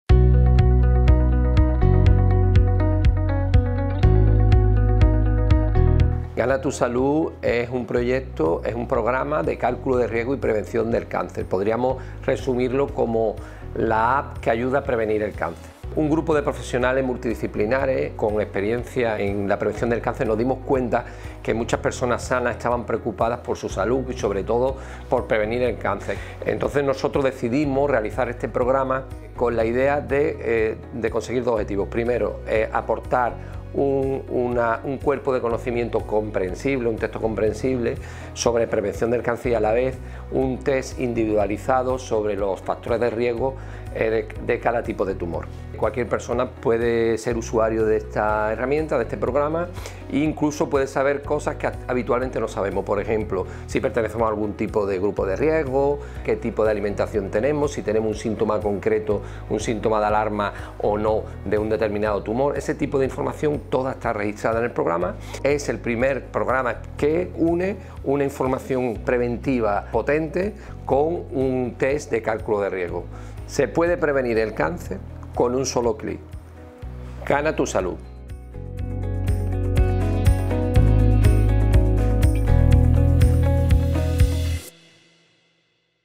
Gana tu salud - zinkinn.es
Gana tu salud es un programa destinado a la prevención contra el cáncer. Un grupo de profesionales con experiencia en la prevención del cáncer nos dimos cuenta de que muchas personas sanas estaban preocupadas e interesadas en prevenir del algún modo el cáncer.

Por ello, pensamos en desarrollar este programa con dos objetivos:

Aportar un cuerpo de conocimientos comprensibles sobre prevención del cáncer y un test individualizado sobre factores de riesgo en cada tipo de tumor.

Cualquier persona puede usar este programa, e incluso aprender cosas nuevas, como descubrir si pertenecemos a un grupo de riesgo, o si tenemos síntomas de urgencia.

Con gana tu salud podemos prevenir el cáncer en un sólo click